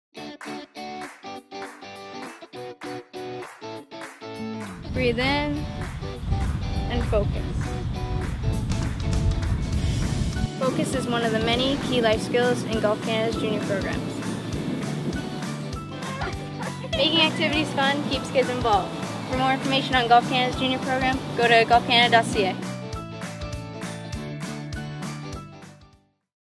Brooke Henderson explique l’une des nombreuses leçons de compétences de vie
En 2015, Golf Canada et la PGA du Canada ont établi un partenariat avec l’Université d’Ottawa pour développer un modèle Compétences de vie axée sur la programmation junior. L’objectif était de mettre encore davantage l’accent sur les valeurs fondamentales véhiculées par le golf et leur pertinence dans le contexte des relations sociales et entre pairs.